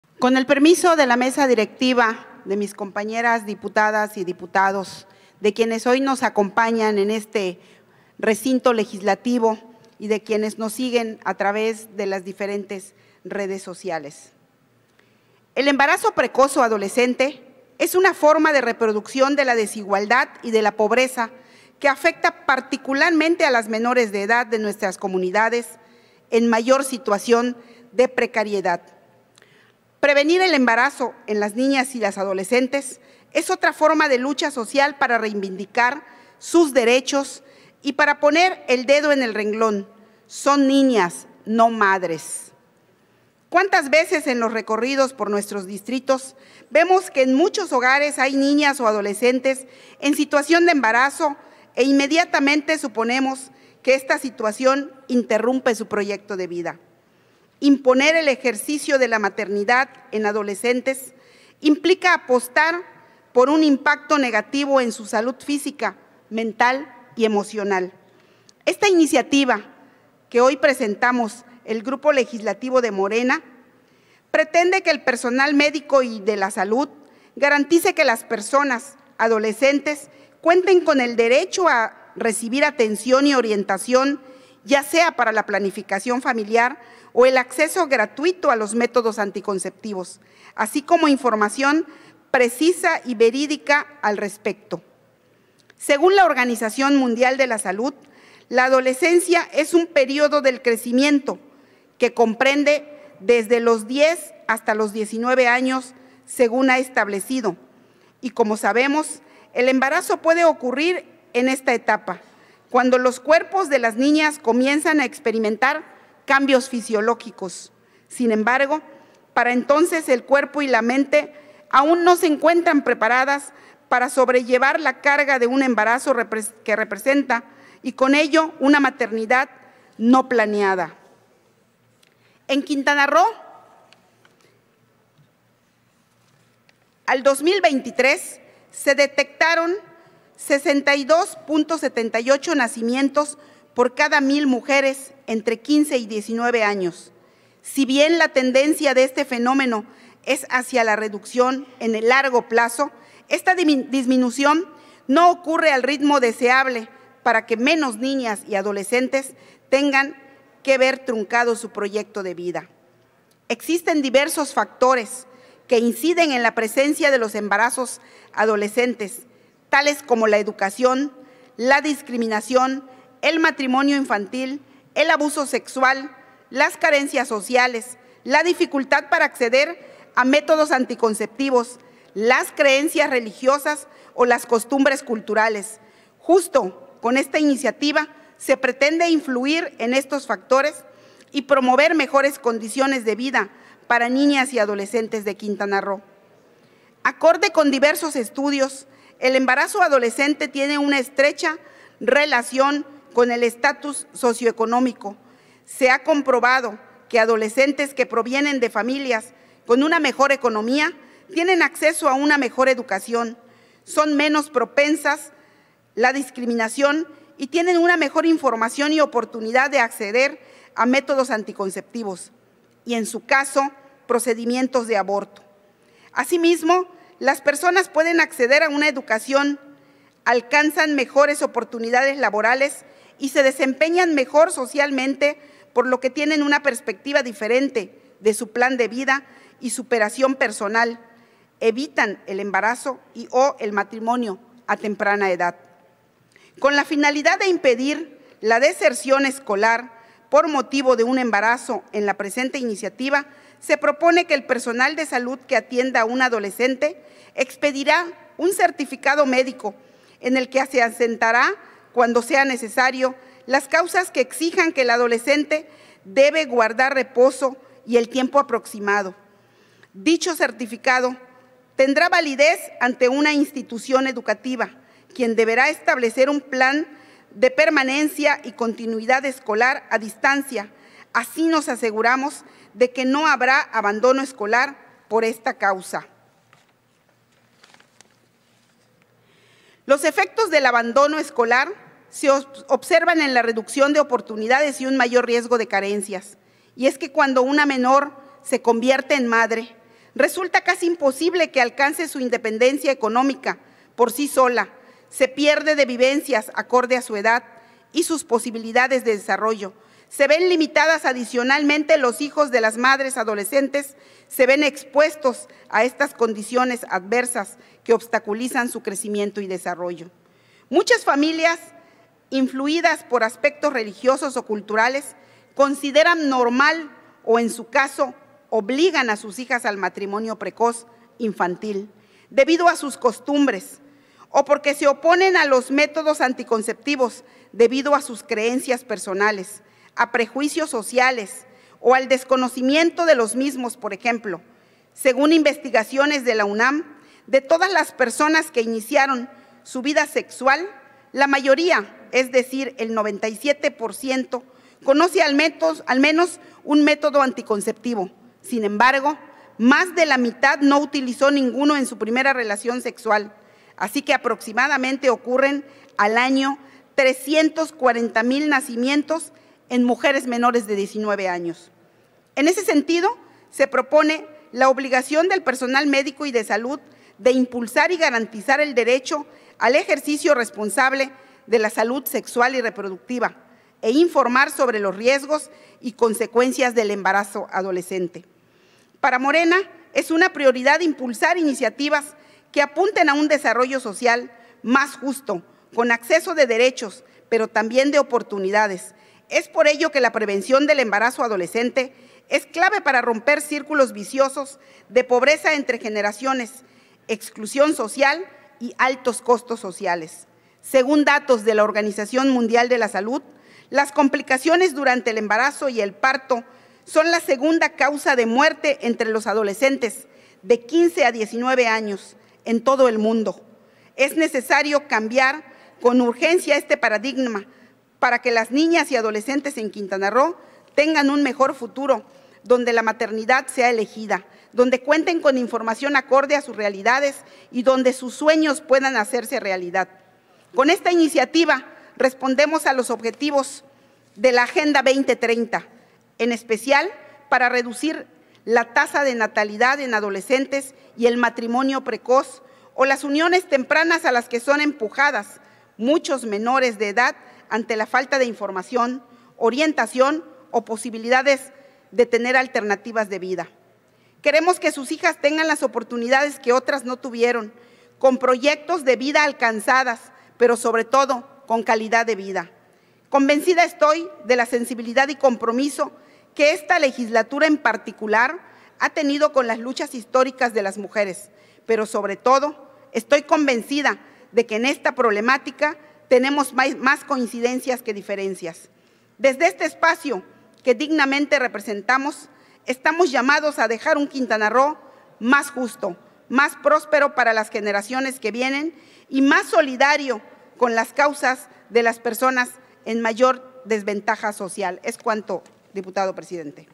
Intervención Dip. Mildred Concepción Ávila Vera. MORENA (16/Octubre/2023)
Intervención de la Dip. Mildred Concepción Ávila Vera, de MORENA, en relación a la Iniciativa de Decreto por el que se reforman y adicionan diversas disposiciones a la Ley de Salud del Estado de Quintana Roo, en materia de embarazo adolescente. Publicada en la Gaceta Parlamentaria Año II número 11 Ordinaria del 12 de octubre de 2023.